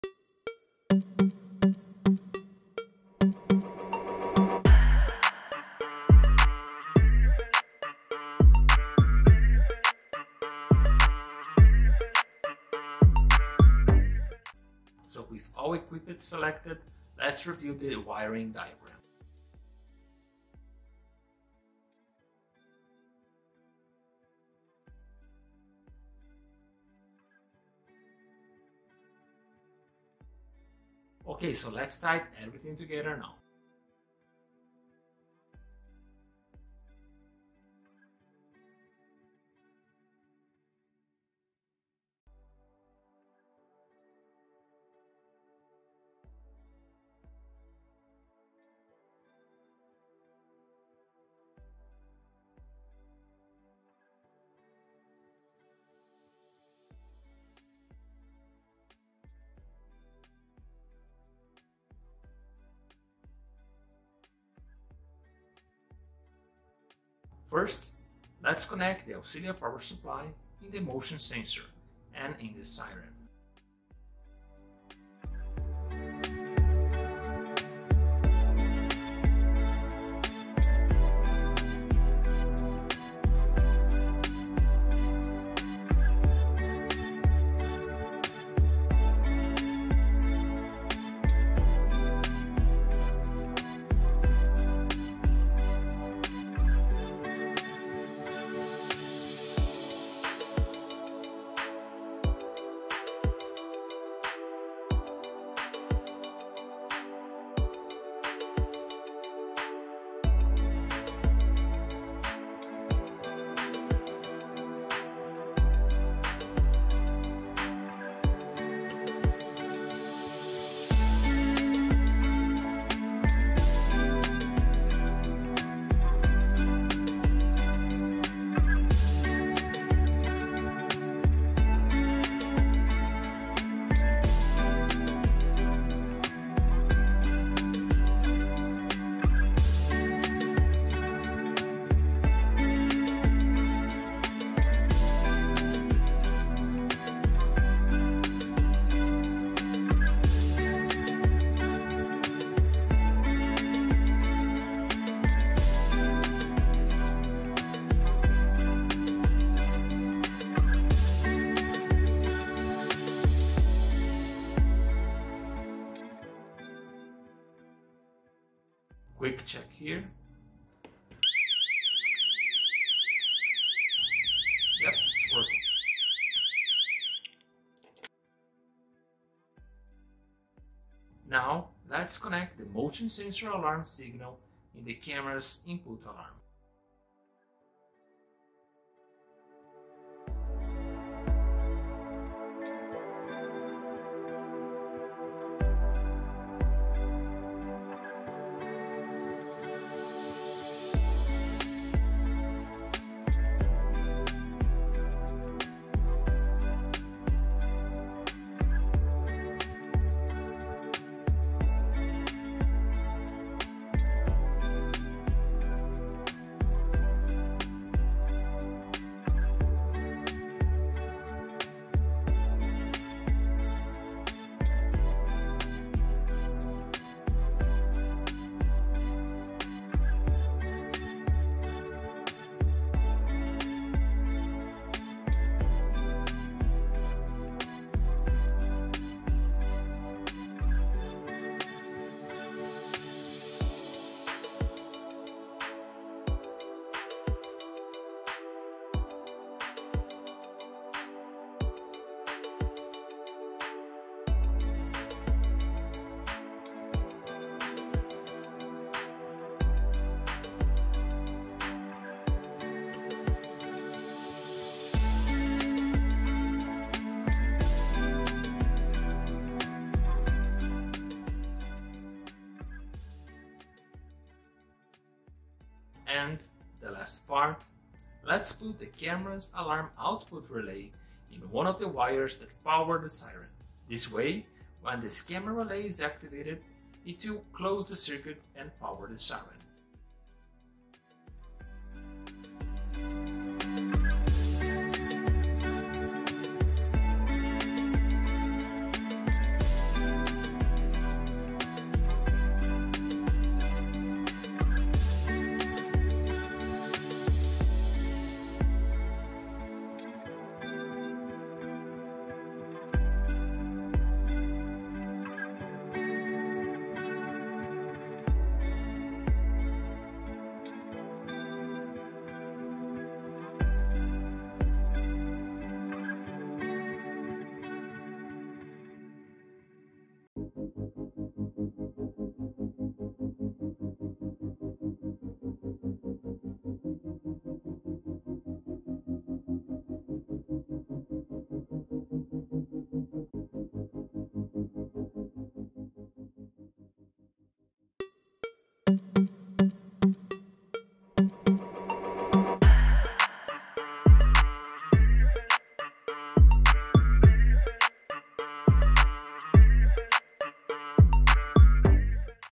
Protecting your perimeter camera - Part 2 - Installation
In this hands-on project, we'll learn how we can use the camera's input and output alarms (I/O) to create simple, low cost and standalone solutions to your video installation.